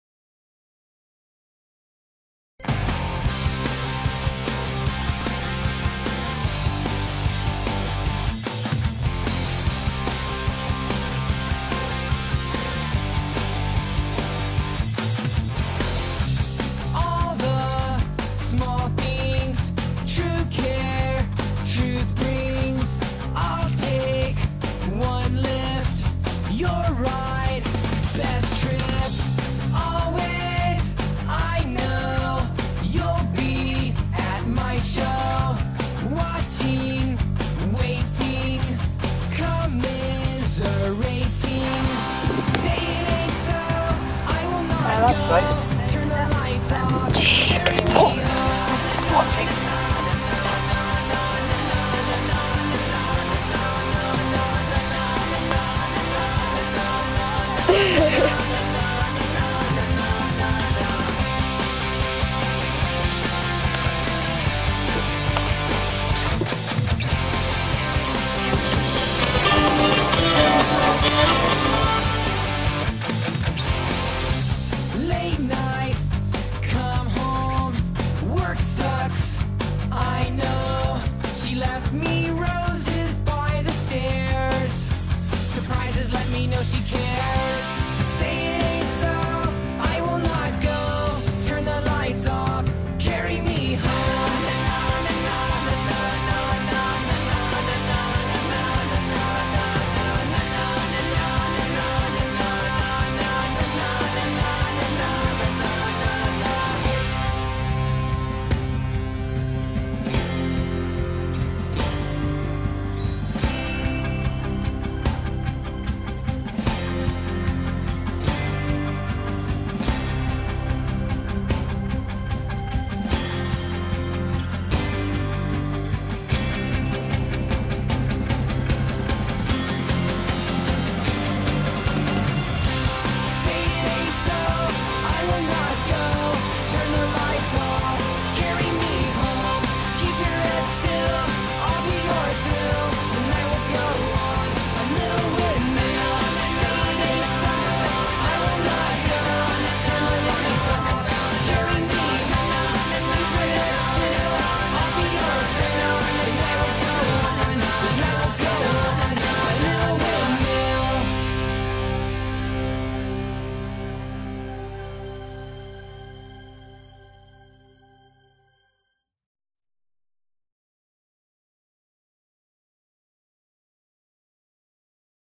Jakey Rakey My lhasa apso pup
Jake aged six weeks to seven months :)
With music from blink 182
Its my first video with windows movie maker so its gonna be rough round the edges :]]